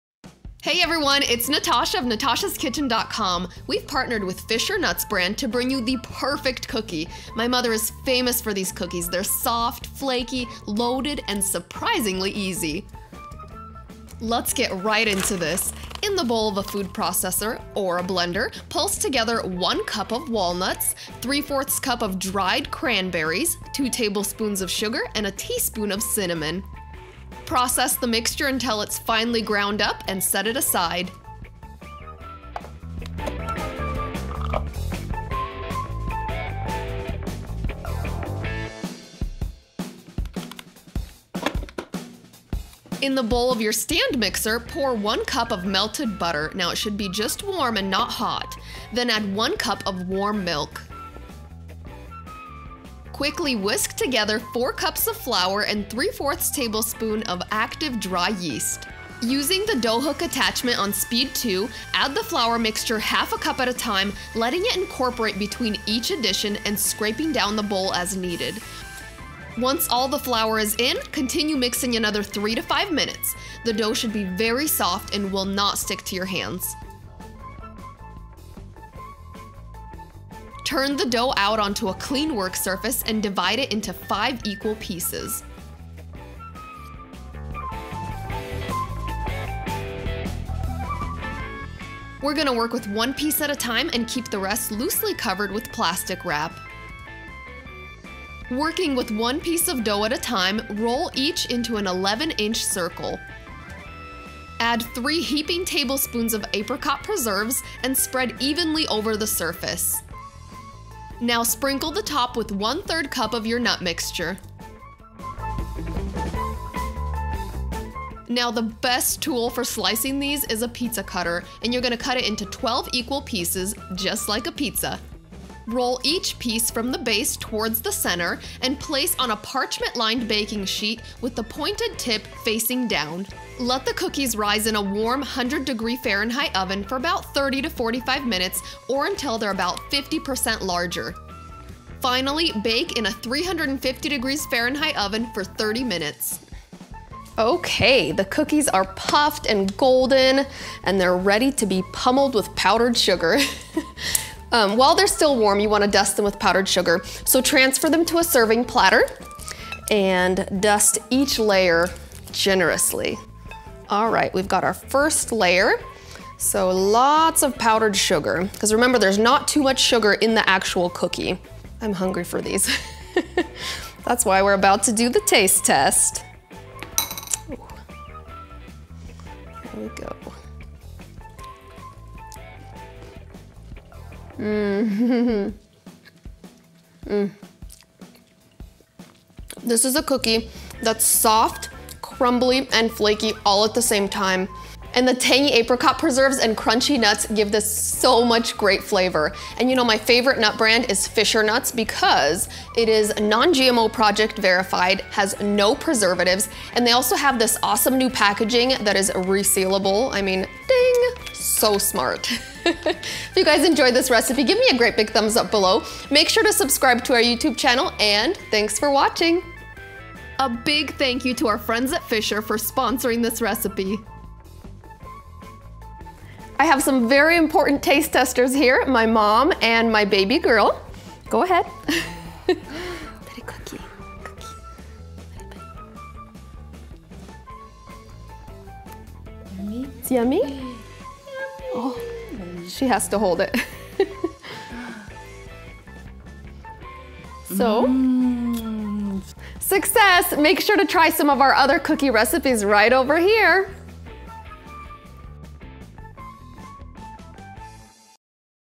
Easy Walnut Rugelach Filled Pastry Recipe | Filled Cookies
These walnut rugelach are soft, crumbly, and perfectly flaky! Also known as ‘rogaliki,’ these cookies are surprisingly easy to make, and the unique cookie dough will amaze you.
⬇️⬇️⬇️⬇️ RECIPE BELOW  ⬇️⬇️⬇️⬇️

Ingredients for Walnut Rugelach Recipe:
►1 cup (16 Tbsp) unsalted butter, melted (not hot)
►1 cup (8oz) warm milk (2% or whole milk)
►4 cups (500 grams) all-purpose flour, measured correctly*
►3/4 Tbsp active dry yeast

Ingredients for Rugelach Filling/ Topping:
►10 oz apricot preserves
►1 cup Fisher walnuts**
►3/4 cup dried cranberries/ craisins, dried cherries, currants, or golden raisins
►Powdered sugar to generously dust cookies

💌Fan Mail:
Natasha's Kitchen